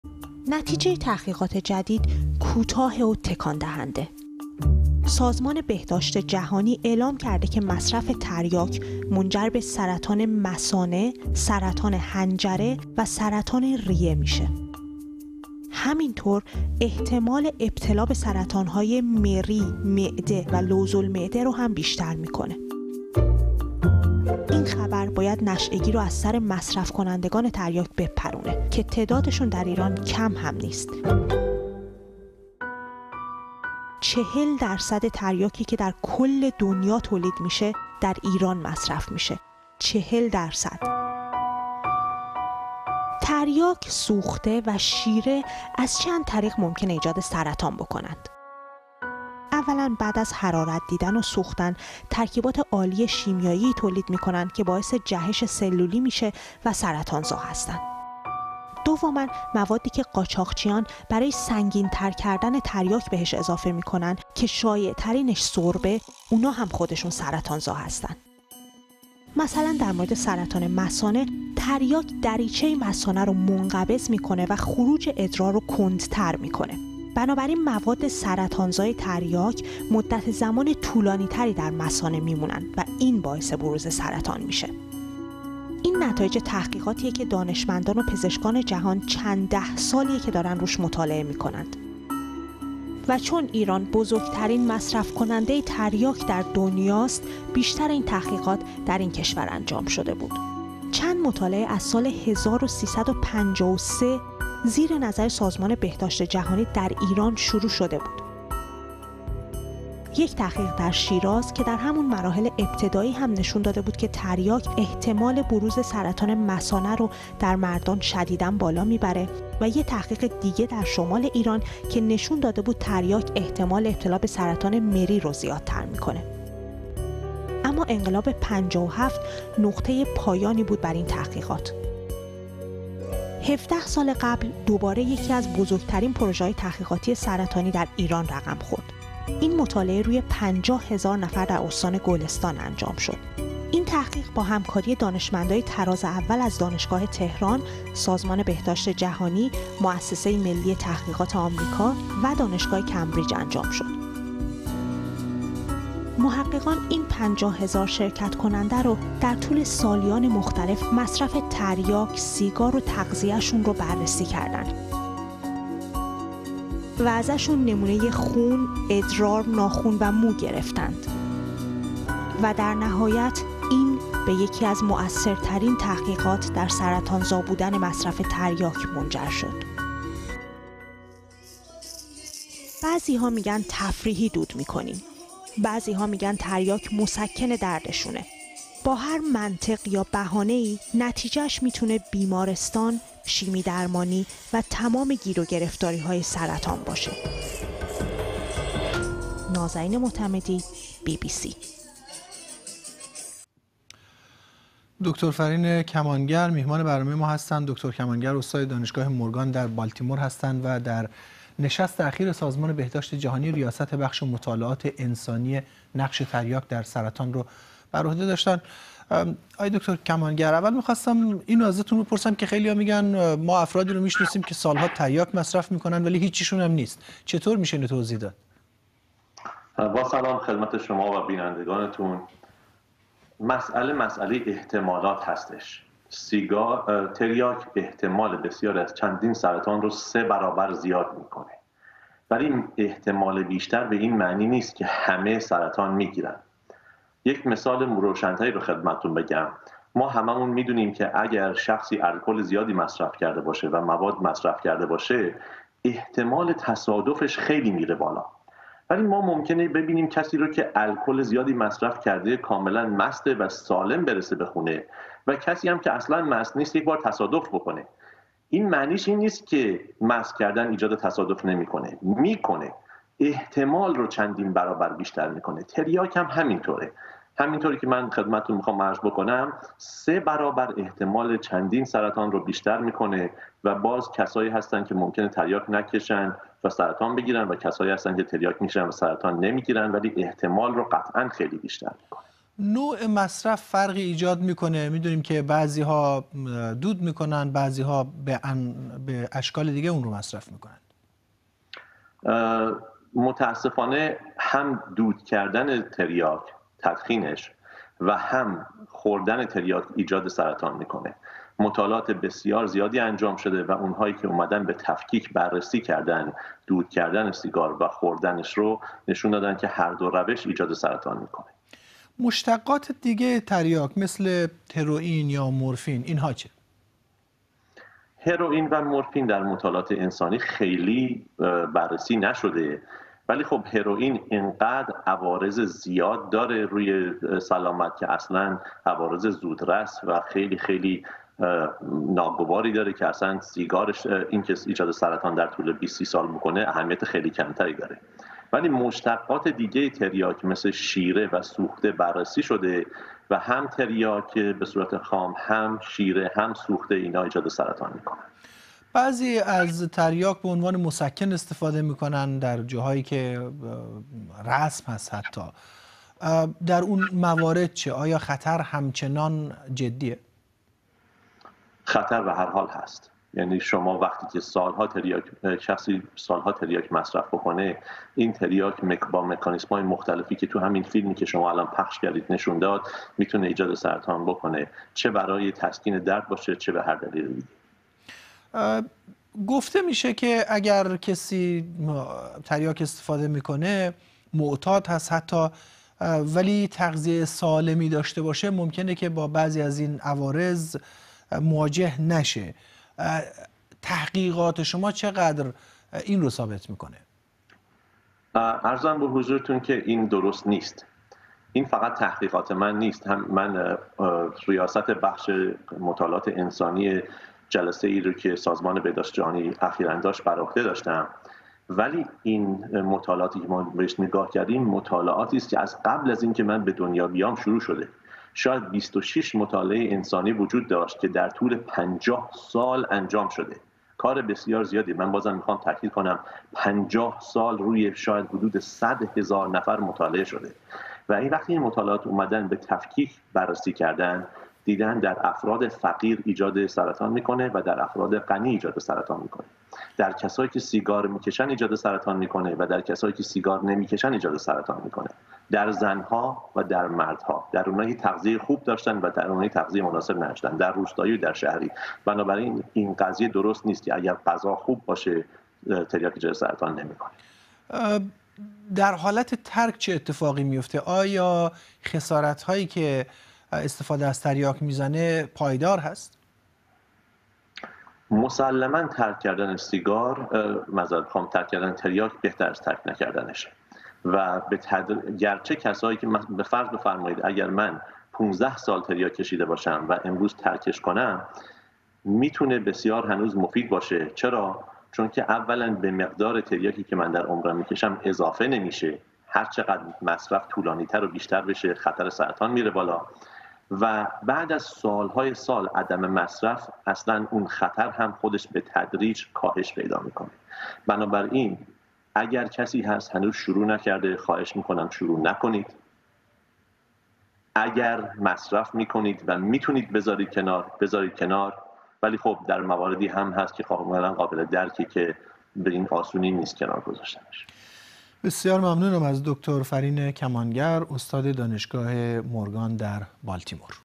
سازمان بهداشت جهانی: مصرف تریاک ممکن است به سه نوع سرطان منجر شود
بزرگترین و معتبرترین موسسات تحقیقاتی دنیا بیش از بیست پژوهش در مورد تریاک را کنار هم گذاشتند و به این نتیجه رسیدند که تریاک نه یکی، ‌نه دو تا بلکه منجر به ایجاد سه سرطان می شود. این خبر برای ایرانیان که ۴۲ درصد تریاک دنیا را مصرف می کند، اهمیت ویژه‌ای دارد.